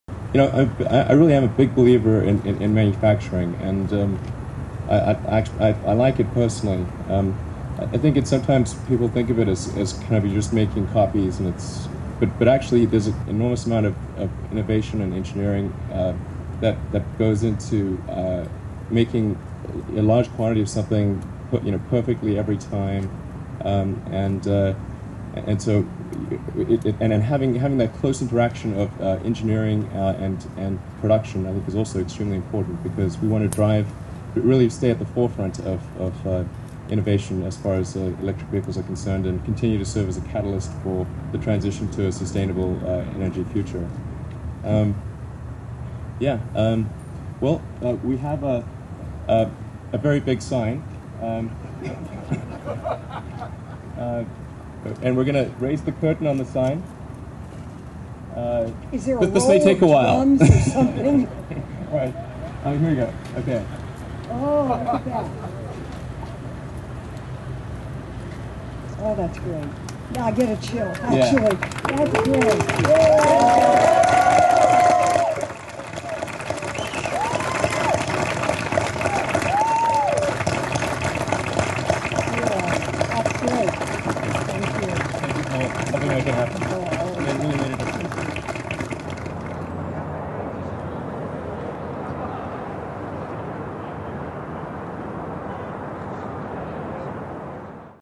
Tesla Opens Factory - Home of Model S
October 27, 2010 Tesla CEO Elon Musk and Senator Dianne Feinstein unveil the sign on the side of the new Tesla Factory in Fremont, CA. The Tesla Factory is the only auto assembly plant in California, and the first facility dedicated exclusively to the mass production of electric vehicles.